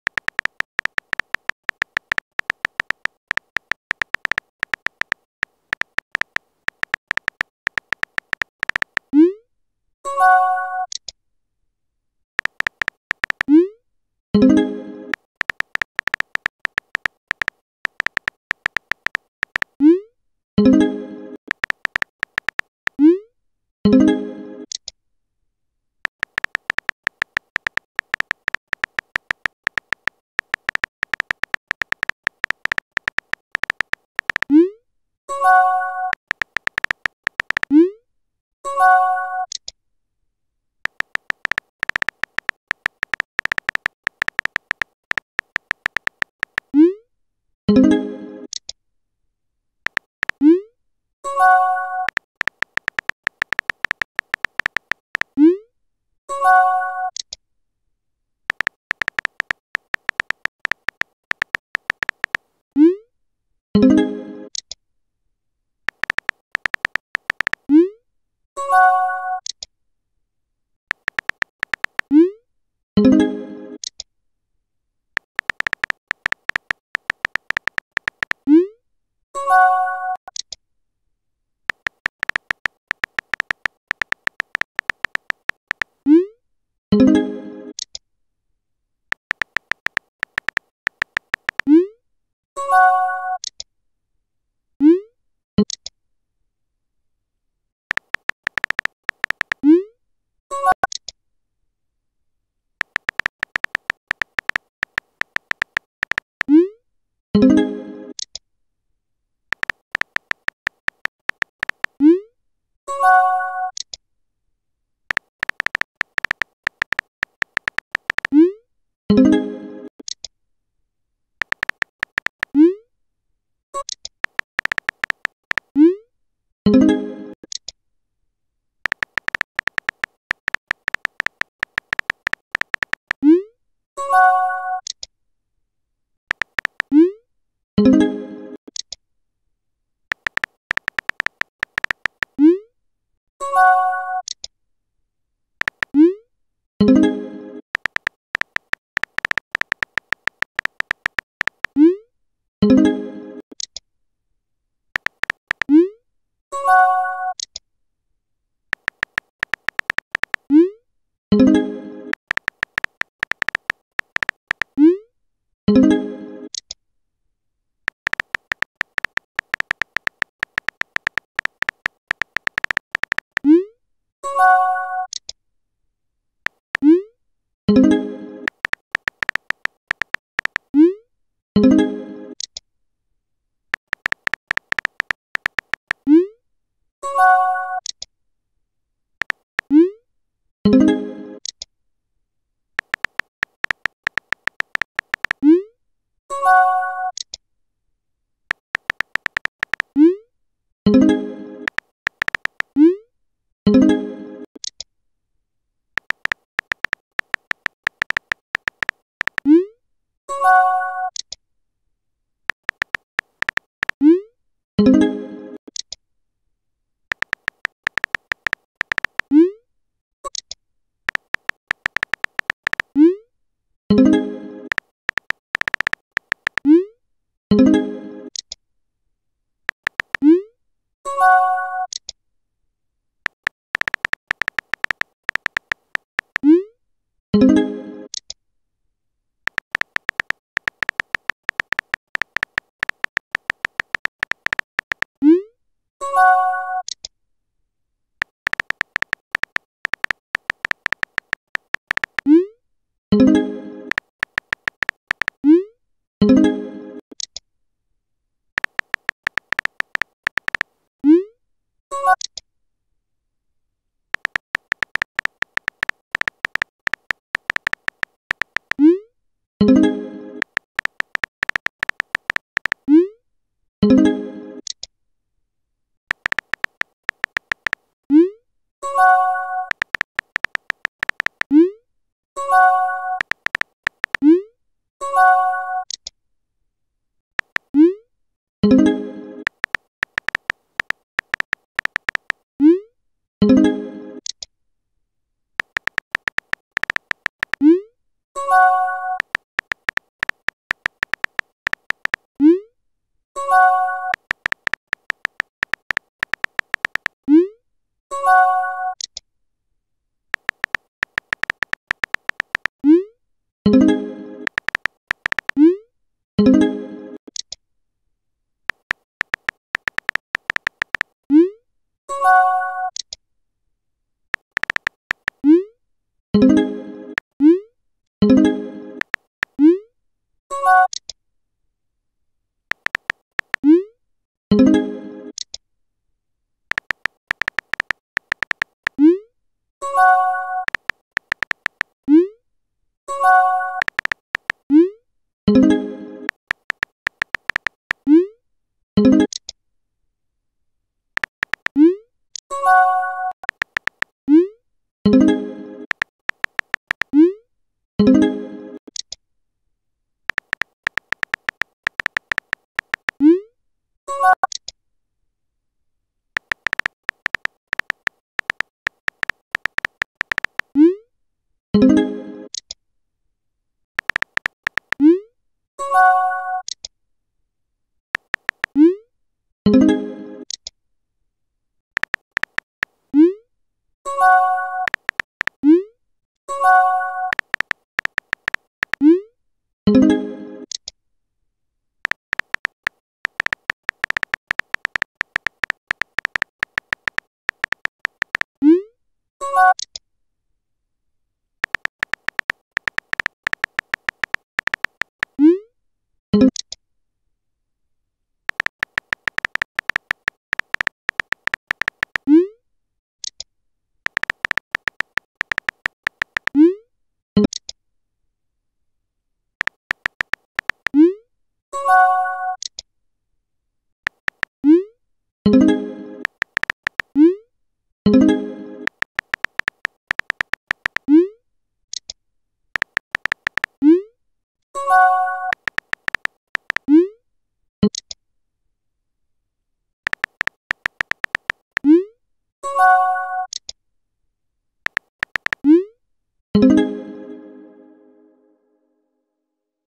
Prinxiety Texting (1) First vid 😀
I may do a part 2 but it's if you guys like it :) Sorry if it's cringe it's my first vid so yay? Inspired by Thomas Sanders - Sanders Sides. Go sub him he is EPIC